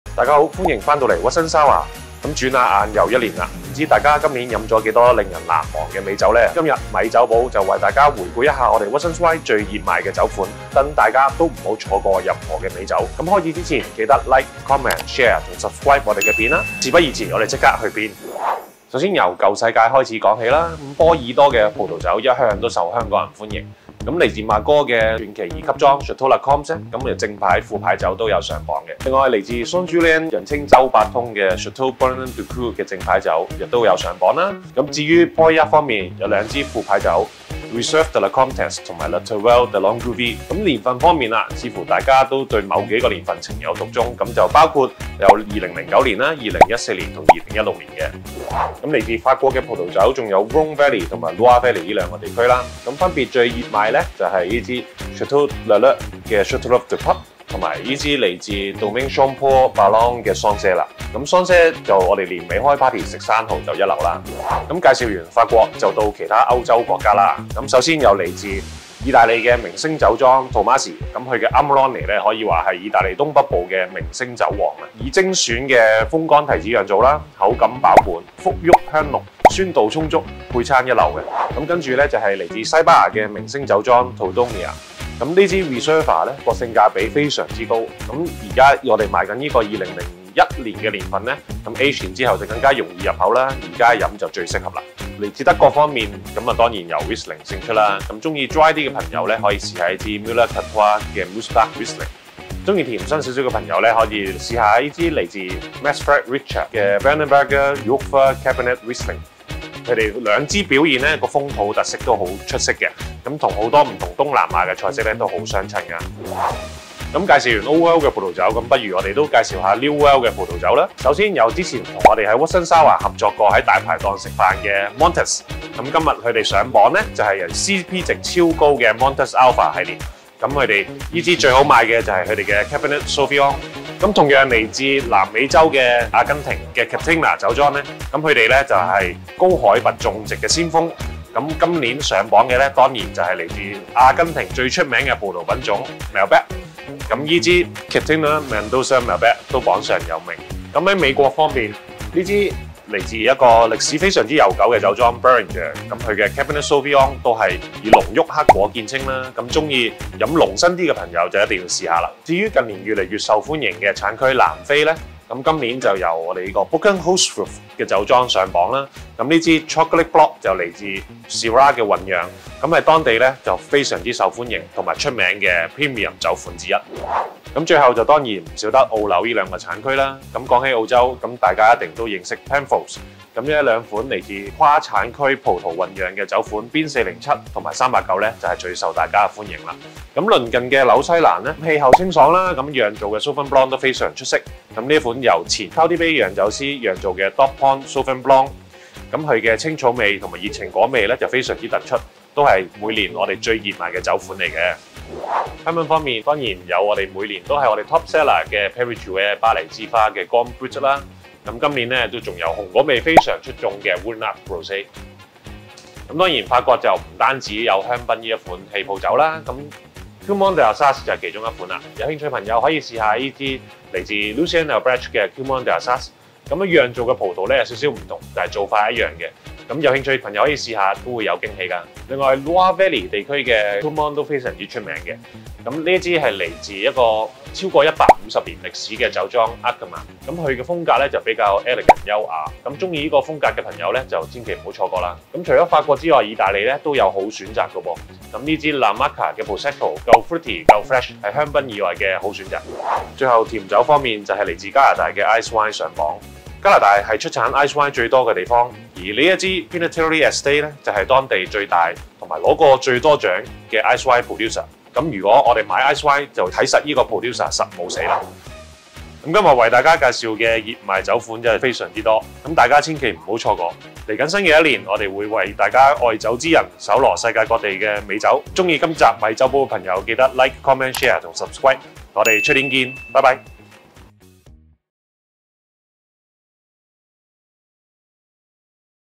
回顧2023年度最愛！盤點25支您最喜愛葡萄酒｜咪酒寶｜Watson’s Hour by Watson’s Wine
轉眼間，2023年尚餘兩星期就會過去，係時候回顧您嘅年度最愛❤️！今集Watson's Hour將會為您盤點本年度25支Watson's Wine最暢銷葡萄酒，當中包括新舊世界嘅紅、白、氣泡酒。即上YouTube睇睇您嘅最愛有無上榜，順便發掘滄海遺珠，把握2023年最後兩星期試盡佢！